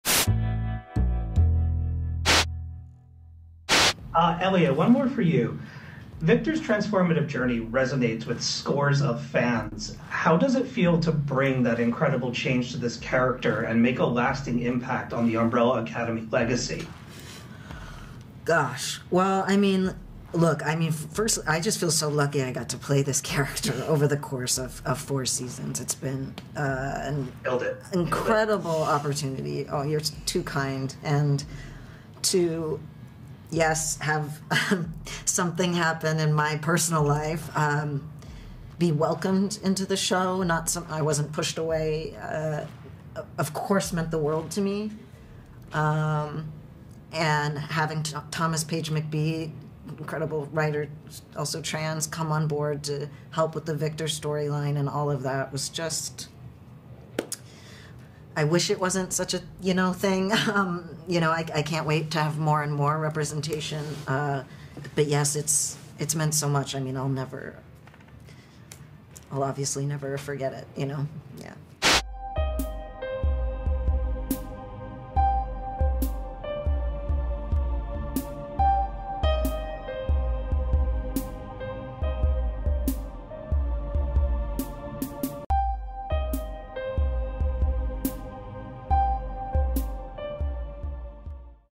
Elliot Page on Victor's transformation in The Umbrella Academy
While promoting the final season of TheUmbrellaAcademy ElliotPage speaks to JoBlo about his character Victor's transformation in the series.